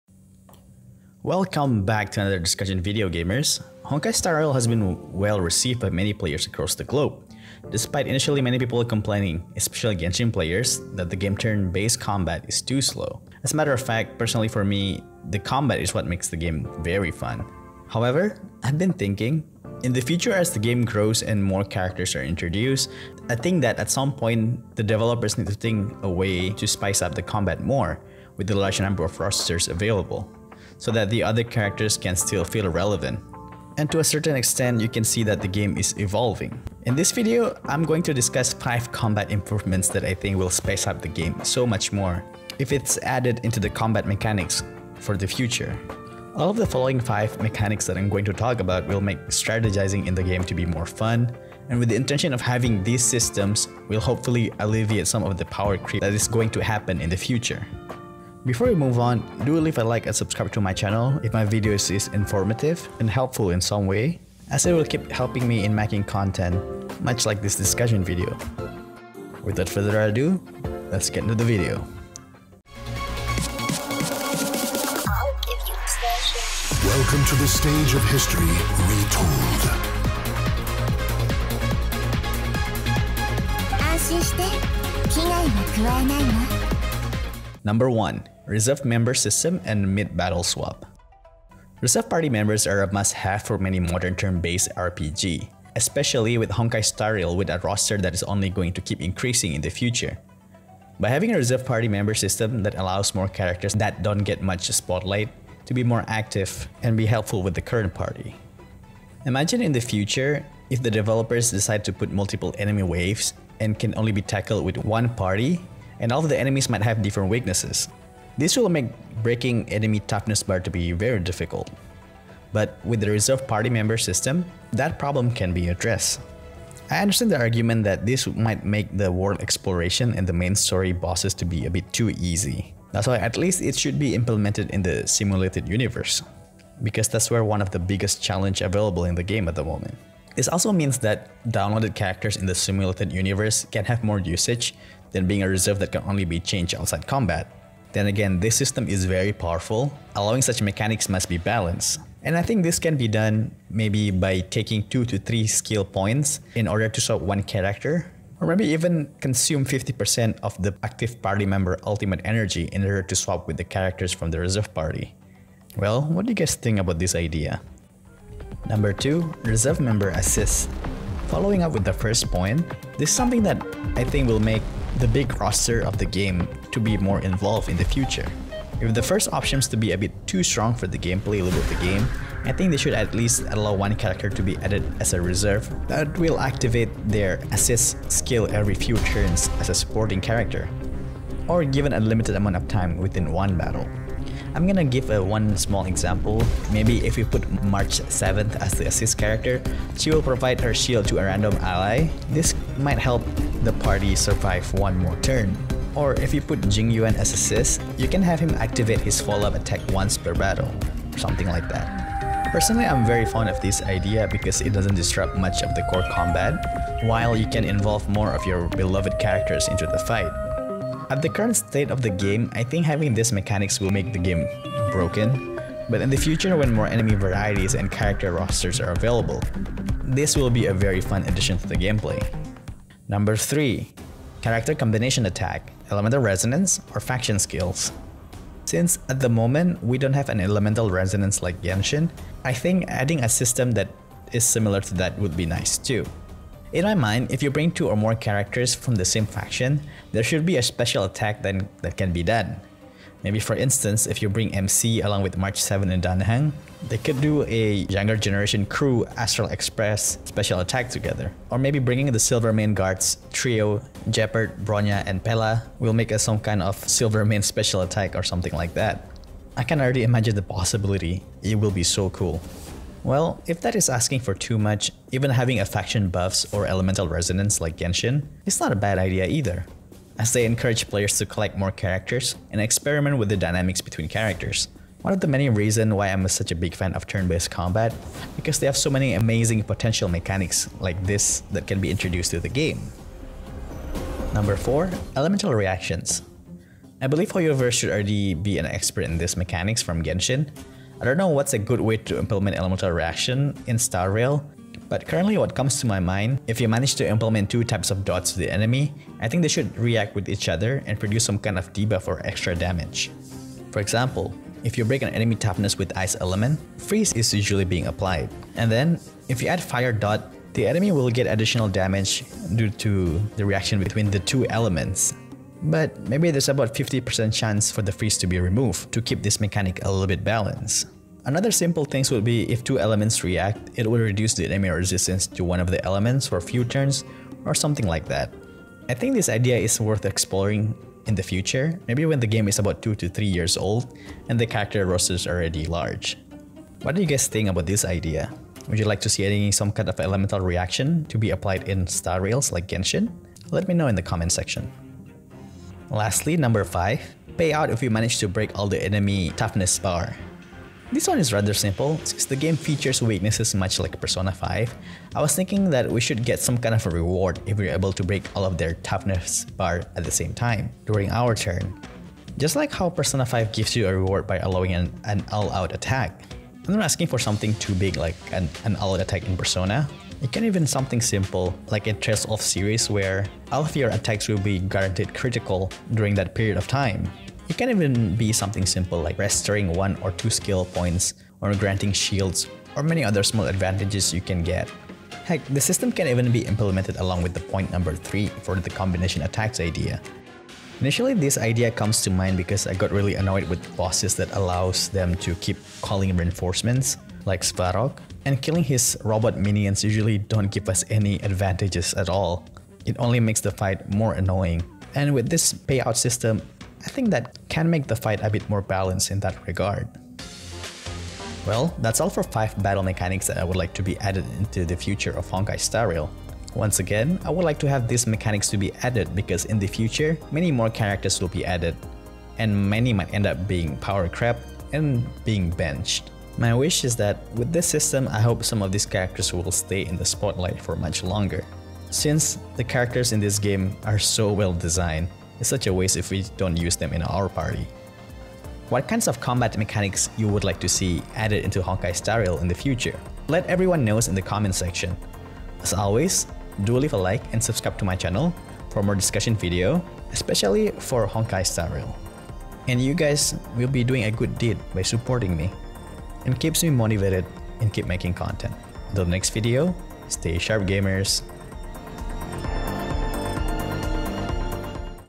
5 Combat Mechanics To Add In Honkai Star Rail | Strive To Be Critical
Honkai Star Rail combat is already fantastic, but I think It can still be spiced up. That's Why I am going to present 5 combat mechanics that I think will spice up the game quite a bit for the future. It is just my hopes and dreams as a turn based RPG geek, and all of the problem with actual combat maybe can be solved by adding more characters, but who knows.

They really do help me out, and I'm always happy to hear from you all and discuss more about your passion in video games. :)
I am looking forward to be able to stream in the future.